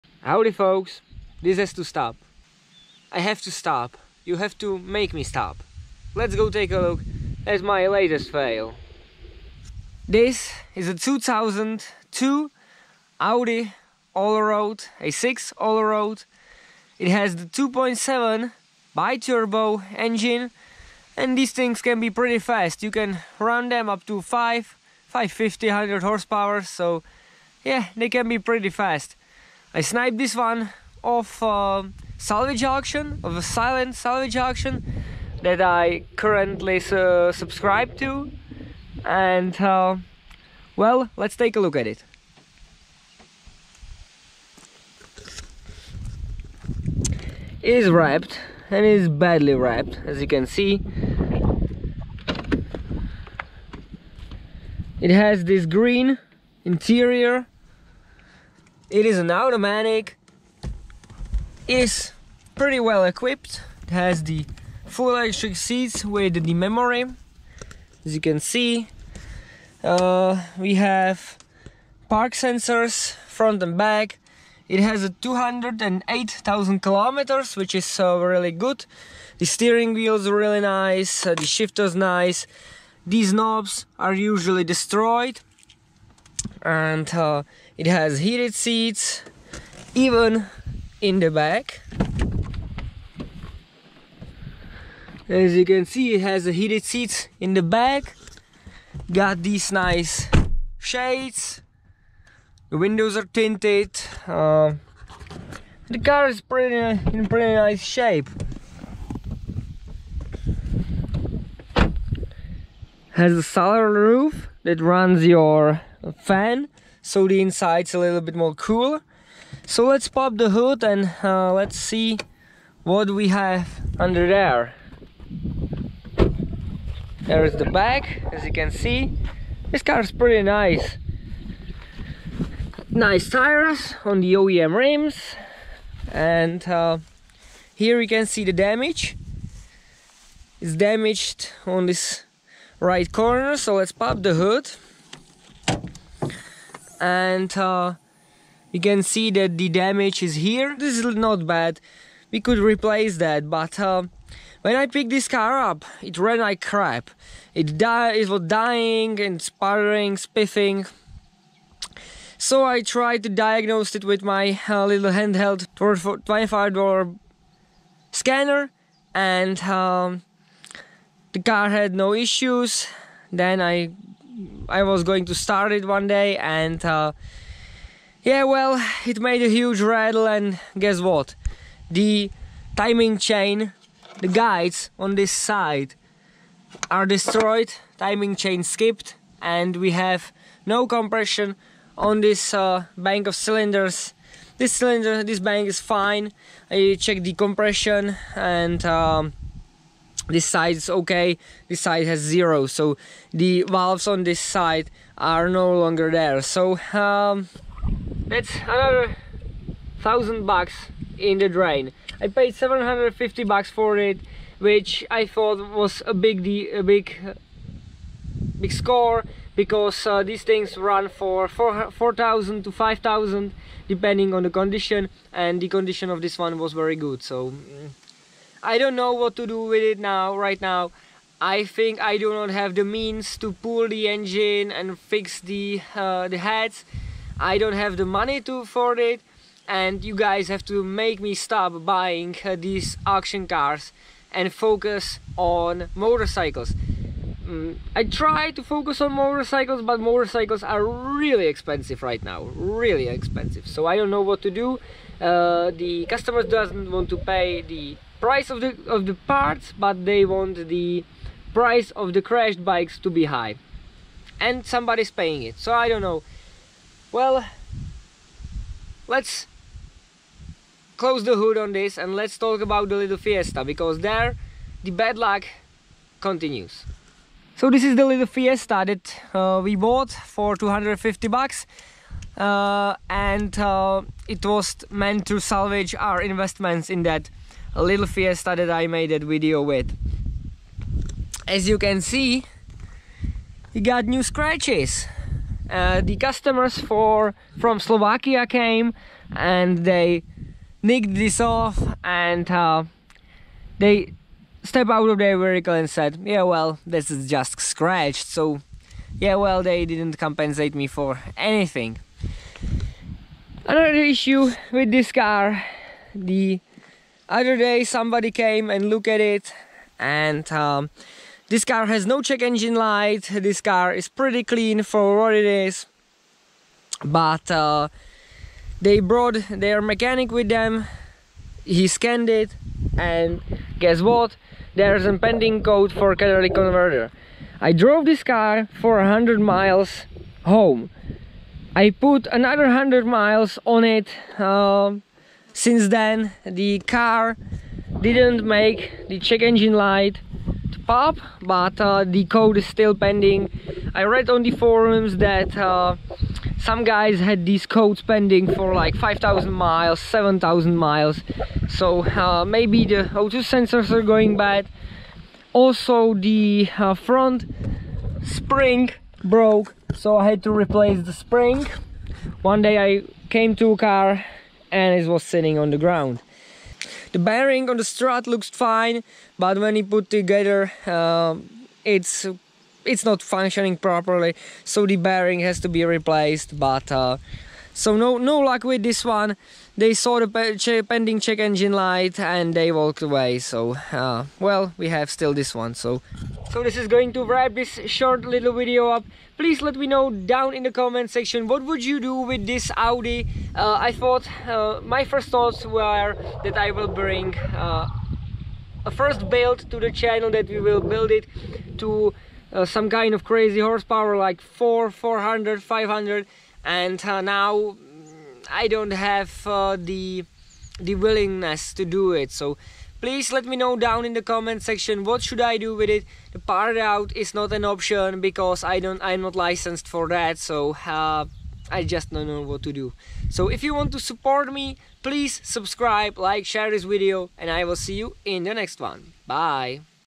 Salvage BI-Turbo Audi FAIL ... Insurance Fraud ?!
Well i bought this audi from the insurance auction.
This car has to run like crap and then granated on me completely.